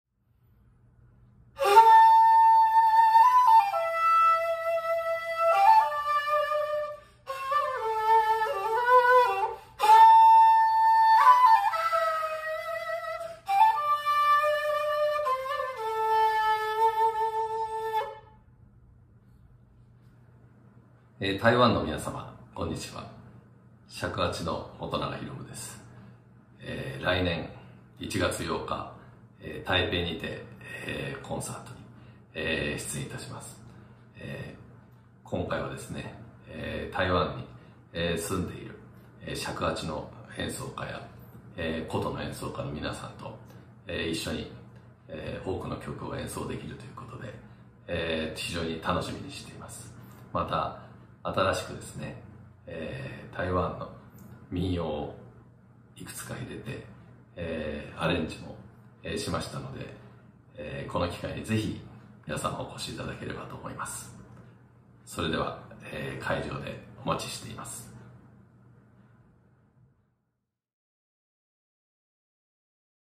華之和音「萌春」音樂會預告｜特邀演出「元永拓」
🔜 華之和音與邦樂大師系列音樂會【萌春】
音樂會特邀演出「元永拓」大師和臺灣打招呼囉 👋 距離音樂盛宴不到一個月，我們正同心協力地準備給大家一個美好的音樂饗宴，還沒購票的您，千萬別錯過這個瞭解日本文化的機會喔！我們在國家演奏廳等待您的蒞臨 🙇‍♀️🙇‍♂️🙇🏻

時間｜2023年 1月 8日（週日）14:30開演
地點｜國家兩廳院演奏廳
購票敬請前往👉
✨✨團體購票，單筆訂購10張以上享8折優惠✨✨